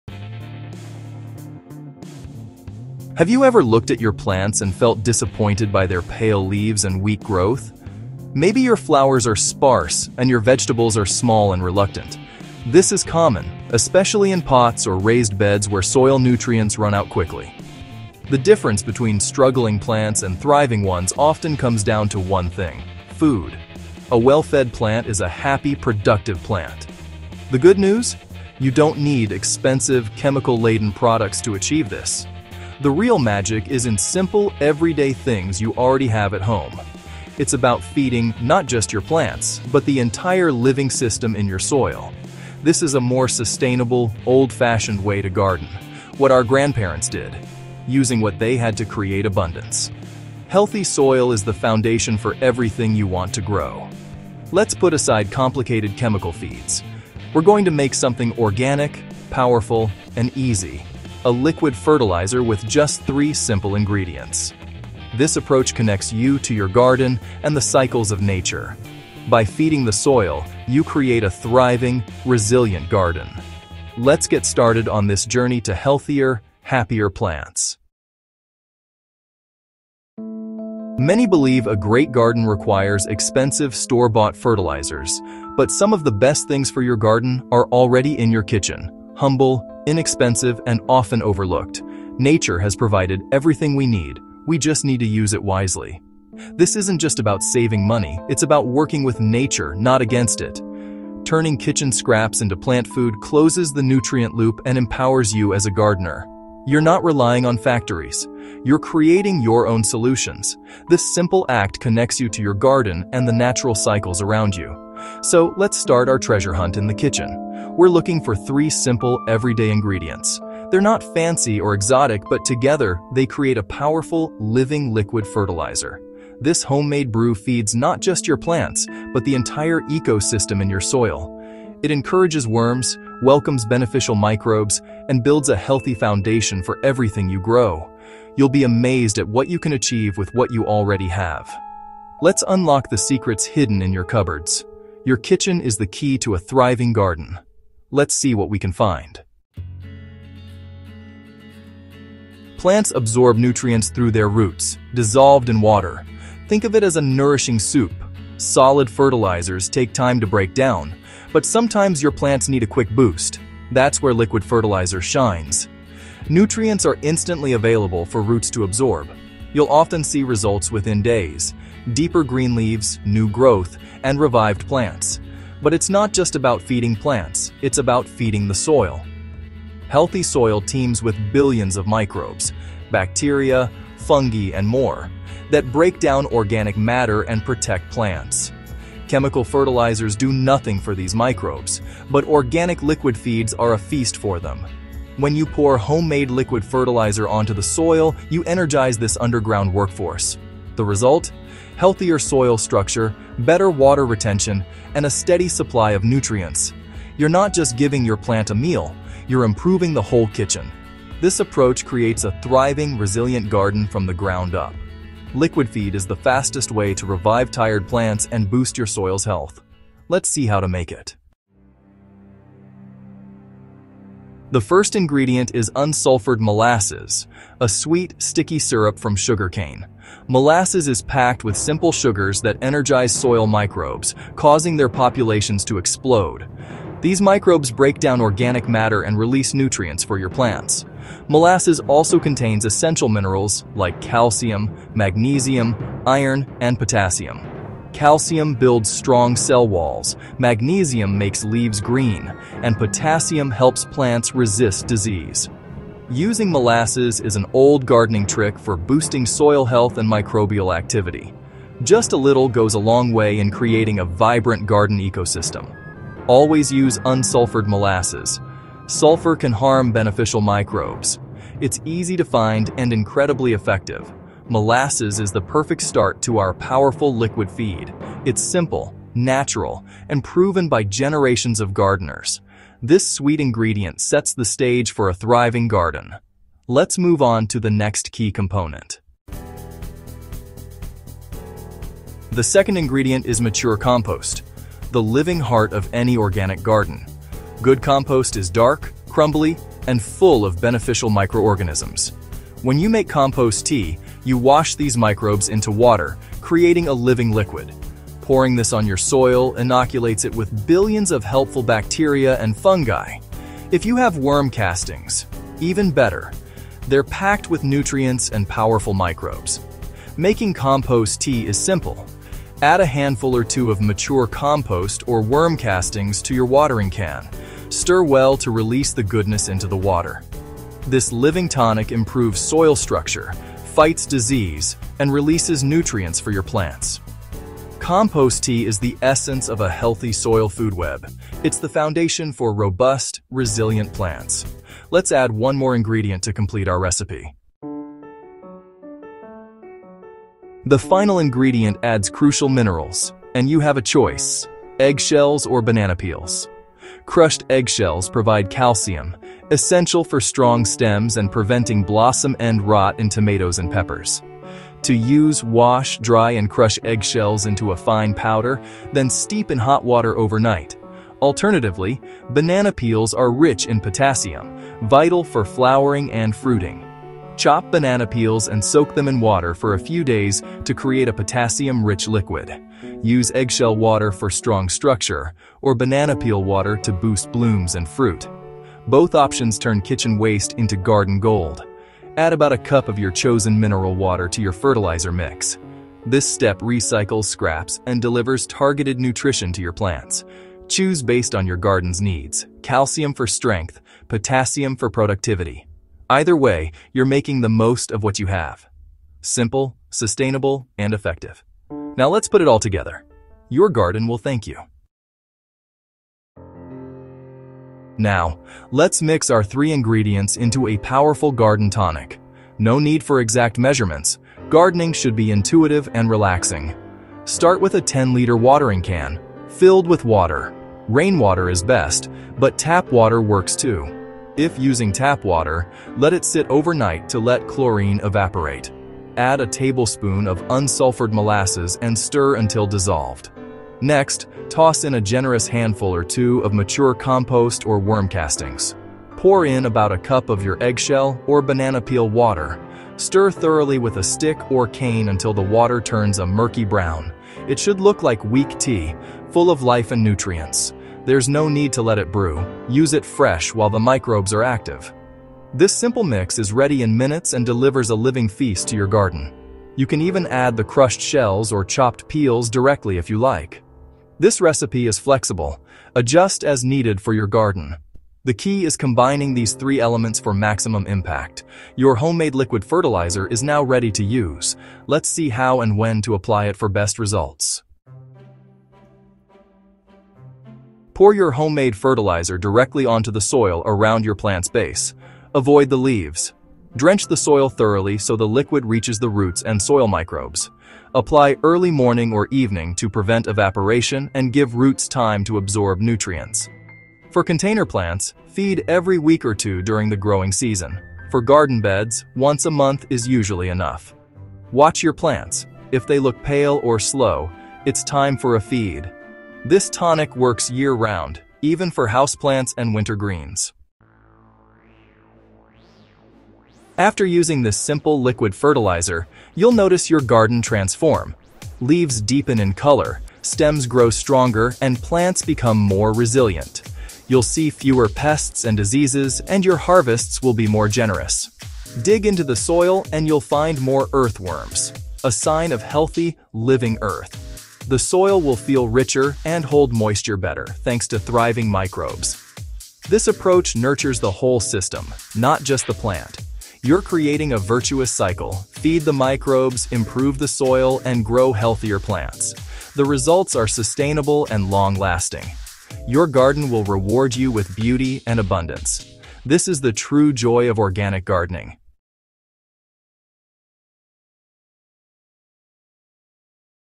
3-Ingredient Liquid Fertilizer That Makes Plants Take Off (Cheap + Organic)
Most gardeners think plants need expensive fertilizers to grow fast — but the truth is, soil life does the real work.

In this video, I reveal a simple 3-ingredient liquid fertilizer that activates soil microbes, feeds plant roots instantly, and works even when dry or cold conditions slow everything else down. This method is fully organic, budget-friendly, and backed by real soil biology — not gardening hype.

Liquid fertilizers work differently from granular feeds. Instead of waiting weeks to break down, liquids deliver nutrients immediately while stimulating beneficial bacteria and fungi already present in your soil. Research from agricultural extension services and soil ecology studies consistently shows that microbial activity is the key driver behind nutrient availability, plant resilience, and earthworm recovery.

You’ll learn:
• Why liquid feeding wakes up “dead soil”
• How microbes unlock nutrients already trapped in your garden
• The exact mistake gardeners make that ruins liquid fertilizers
• Why earthworms return when soil biology is restored
• How to apply this safely without burning plants

This method works for raised beds, containers, backyard gardens, and even indoor plants. It’s especially effective during cooler seasons when traditional fertilizers underperform.

If you’re serious about organic gardening, living soil, and long-term soil health — this is one technique you’ll use again and again.

👇 Don’t miss the pinned comment for a critical warning most gardeners ignore.